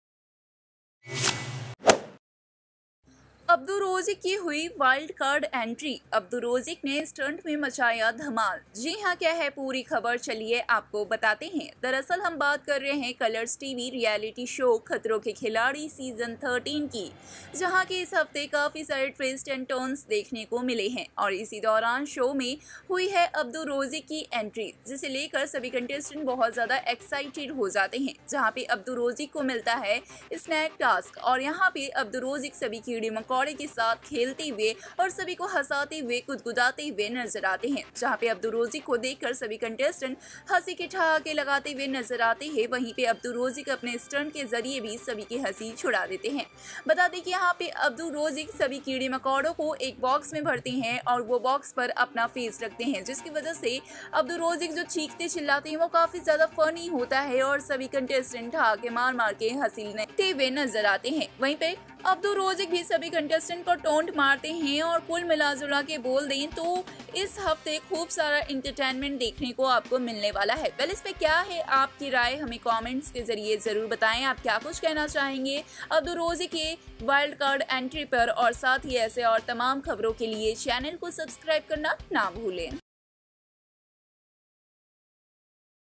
Khatron Ke Khiladi 13 Update: Abdu Rozik Ki Hui Wild Card Entry, Stunt Mein Abdu Ne Machaya Dhamal
Colors TV Show Khatron Ke Khiladi 13 Update: Khatron ke Khiladi Season 13: Abdu Rozik Ki Hui Show Me Wild Card Entry, Stunt Mein Abdu Ne Machaya Dhamal | Colors TV